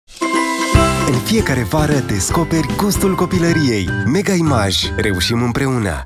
Mega Image | Vara descoperi gustul copilăriei
Când temperaturile cresc, găsești la Mega-ul tău prețuri promoționale la desertul favorit al verii! Te așteptăm pe la noi să celebrăm vara cu înghețata ta preferată. Promoția este valabilă în perioada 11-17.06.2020.